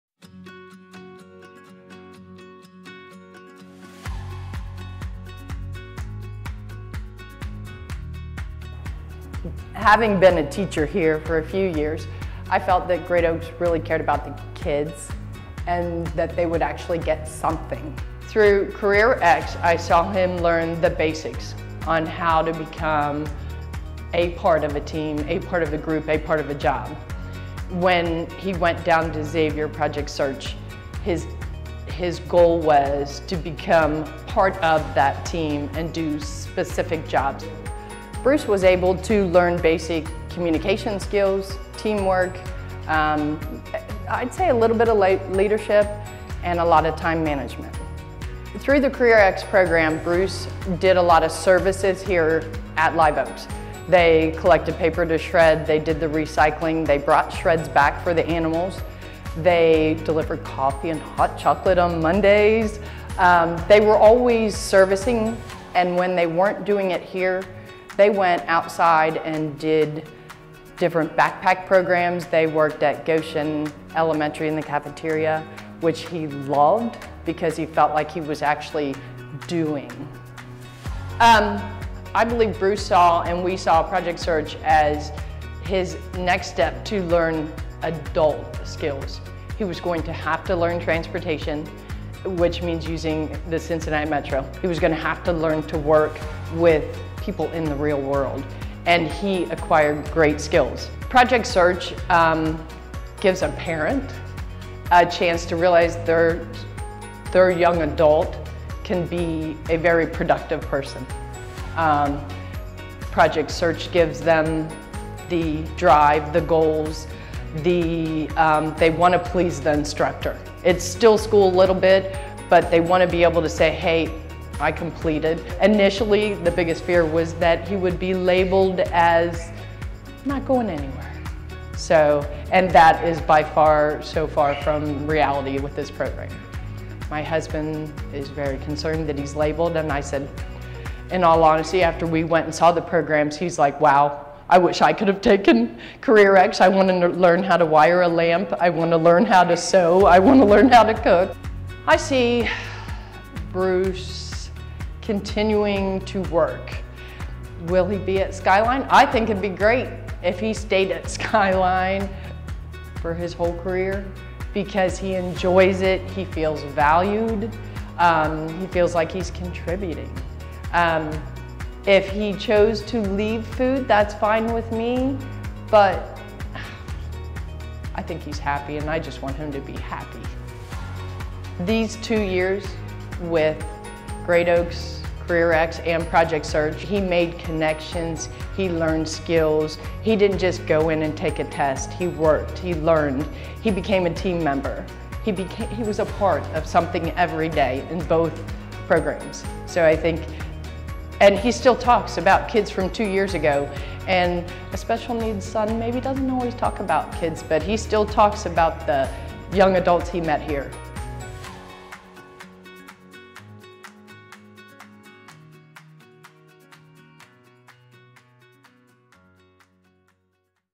Great Beginnings: Kim Brown Shares Her Son's Story from Career X/Project SEARCH
Listen to mom and Great Oaks teacher, Kim Brown share her story of her son's journey in the Career X and Project SEARCH Programs at Great Oaks.